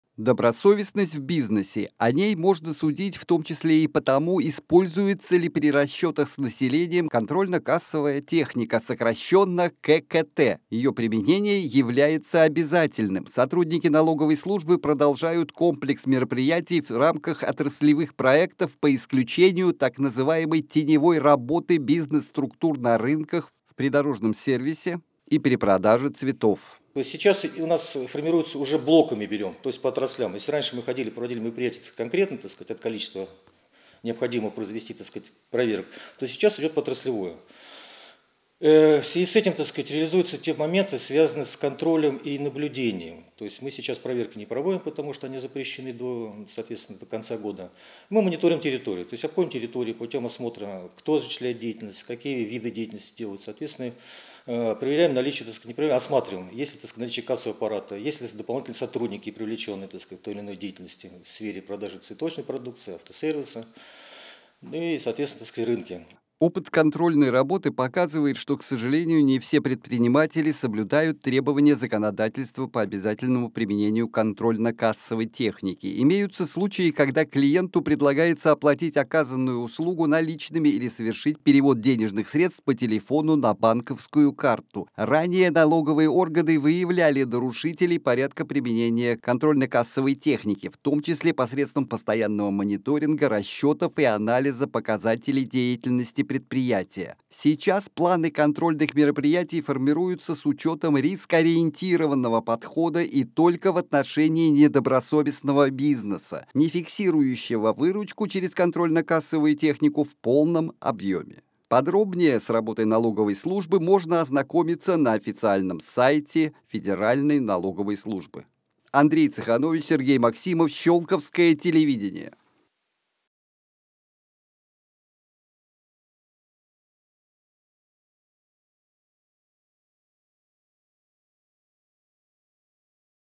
Налоговая служба: применение онлайн-касс обязательно!
Наш налоговый путеводитель. Налоговая служба напоминает: юридические лица и предприниматели, оказывающие услуги населению, обязаны применять онлайн-кассы.